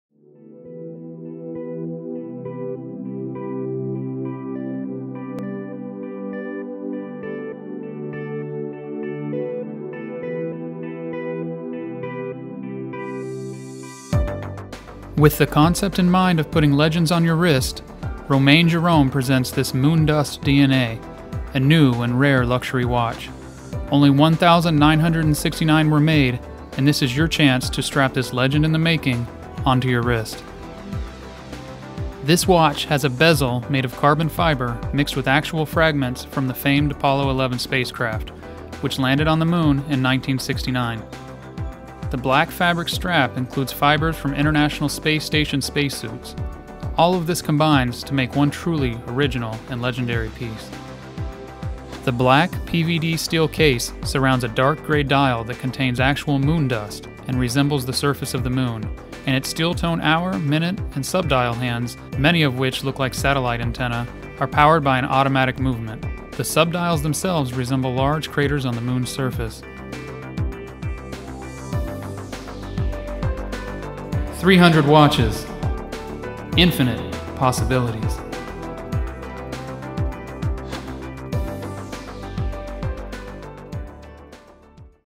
Romain Jerome Moon Dust-DNA PVD Steel Black Mood Watch
This men's Romain Jerome Moon Dust-DNA watch, model RJ.M.CH.001 Black Mood Black, is a new and rare luxury watch — only 1,969 were made. Photos of the actual timepiece show that it is in excellent condition, with no signs of wear on the case, crystal, crown, back plate, or strap. It is fully functional, and guaranteed 100% authentic. The retail price for this watch is $17,300.

This is another case of Romain Jerome bringing the legends of our past into the present. This watch has a bezel made of carbon fiber mixed with actual fragments from the famed Apollo 11 spacecraft, which landed on the moon in 1969. The case is black PVD steel, surrounding a dark-grey dial that contains actual moon dust, and resembles the surface of the moon. The screwed-down PVD-steel back plate shows information about the watch.